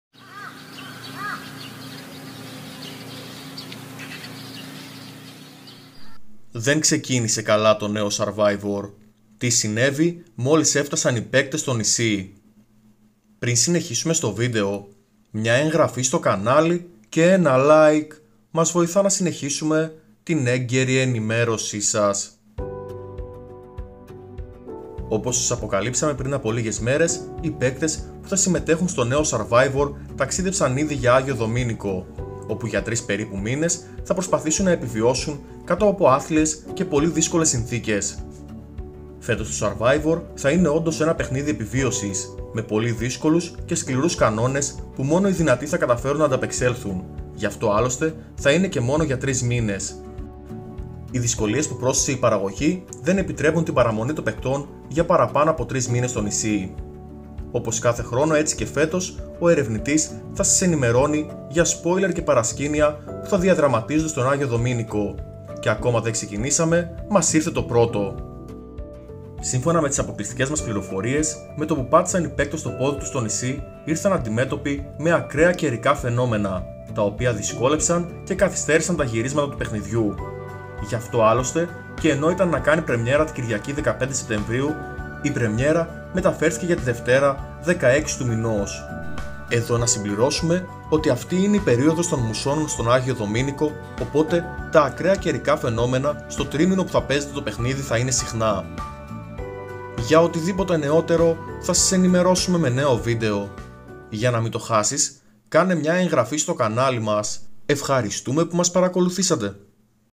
Νέο Survivor: ΔΕΝ ξεκίνησε καλά... Τι συνέβη μόλις έφτασαν οι παίκτες στο νησί!!!
Erevnitis TV - Ερευνητής

Ευχαριστούμε το You weekly gr👍👍